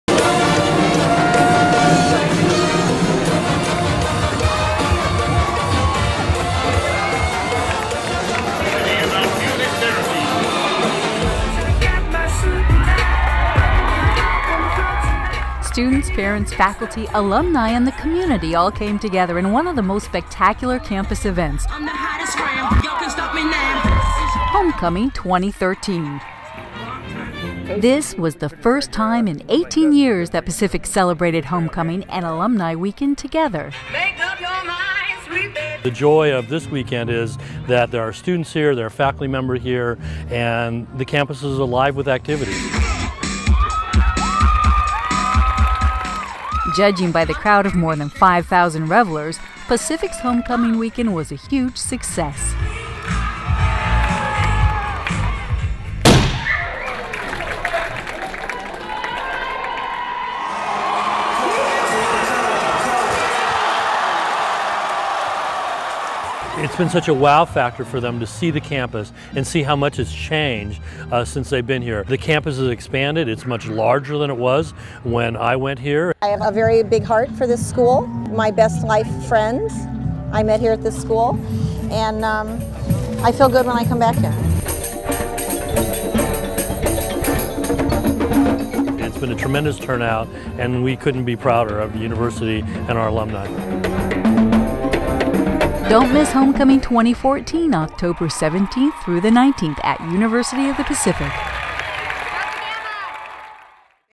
UOP Homecoming 2013
A great success as  UOP  celebrates Homecoming and alumni weekend together for the first time in 18 years.
The campus, alive with activity, hosted more than 5,000 visitors.
Save the date for Homecoming 2014, October 17-19.